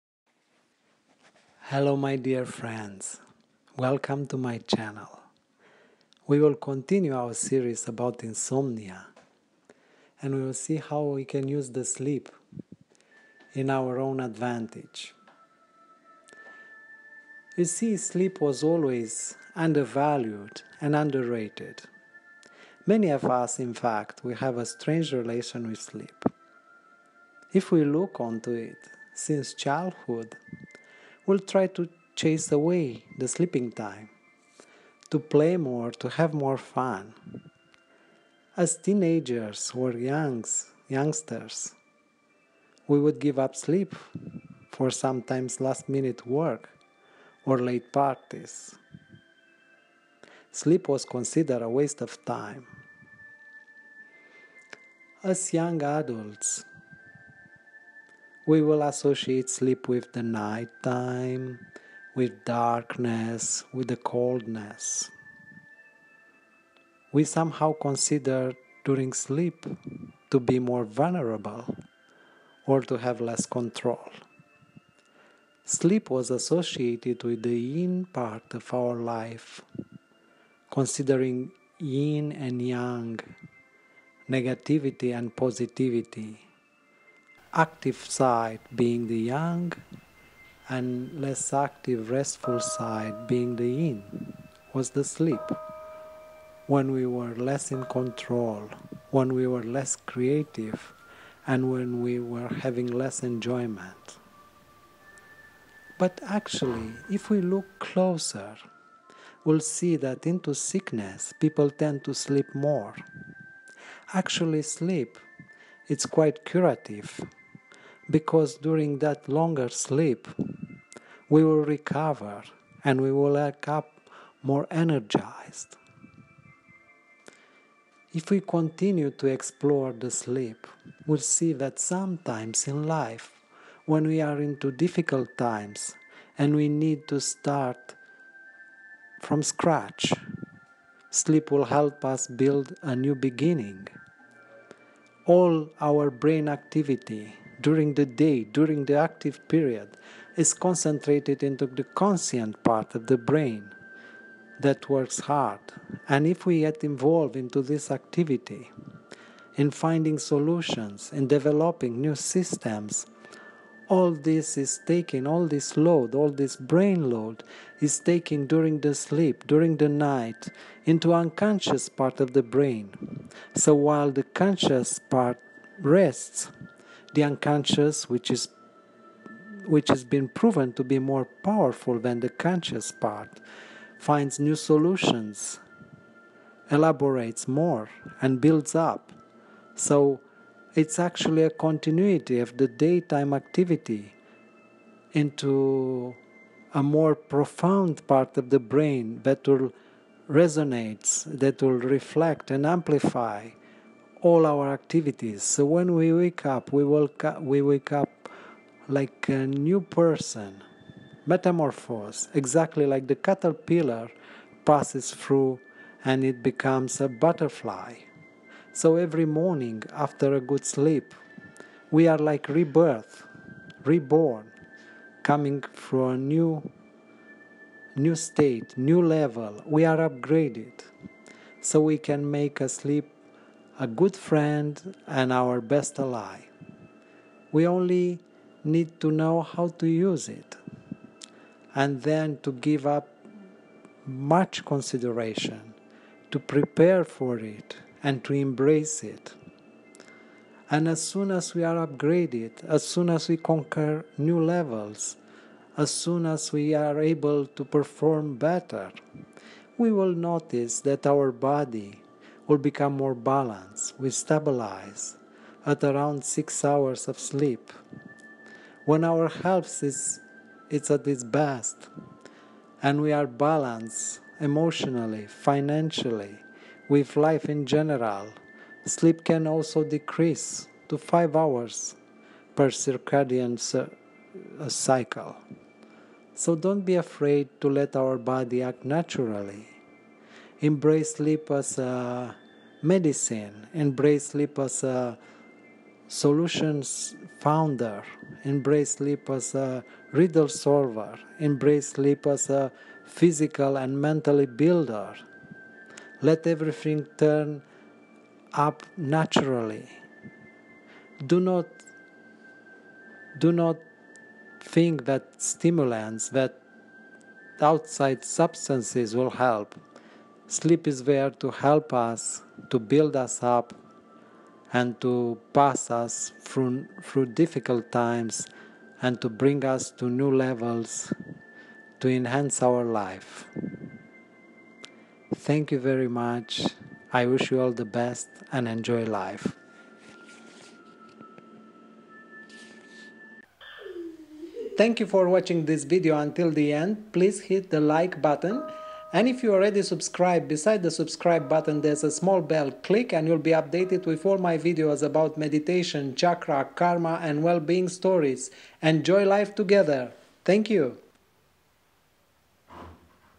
WithMe Insomnia Meditation ASMR (unintentional)
Covering and helping you understand certain aspects about insomnia and sleep in general from a TaiChi perspective.